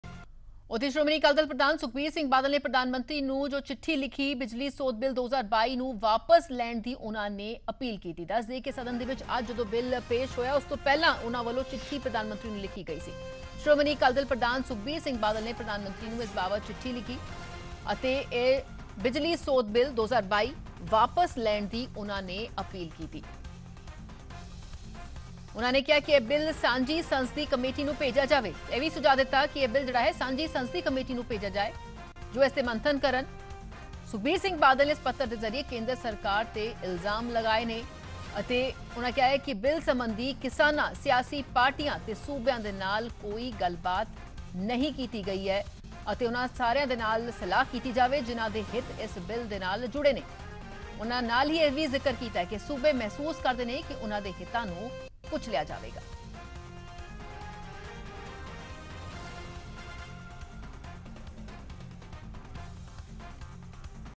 Electricity Amendment Bill 2022: ਸੁਖਬੀਰ ਸਿੰਘ ਬਾਦਲ ਨੇ ਬਿਜਲੀ ਸੋਧ ਬਿੱਲ 2022 ਵਾਪਸ ਲੈਣ ਦੀ ਕੀਤੀ ਅਪੀਲ
ਸ਼੍ਰੋਮਣੀ ਅਕਾਲੀ ਦਲ ਪ੍ਰਧਾਨ ਸੁਖਬੀਰ ਸਿੰਘ ਬਾਦਲ ਨੇ PM ਮੋਦੀ ਨੂੰ ਲਿਖੀ ਚਿੱਠੀ। ਬਿਜਲੀ ਸੋਧ ਬਿੱਲ 2022 ਵਾਪਸ ਲੈਣ ਦੀ ਕੀਤੀ ਅਪੀਲ। ਕਿਹਾ ਇਹ ਬਿਲ ਸਾਂਝੀ ਸੰਸਦੀ ਕਮੇਟੀ ਨੂੰ ਭੇਜਿਆ ਜਾਵੇ।

Live Latest News Punjab  | PTC News Live | Breaking News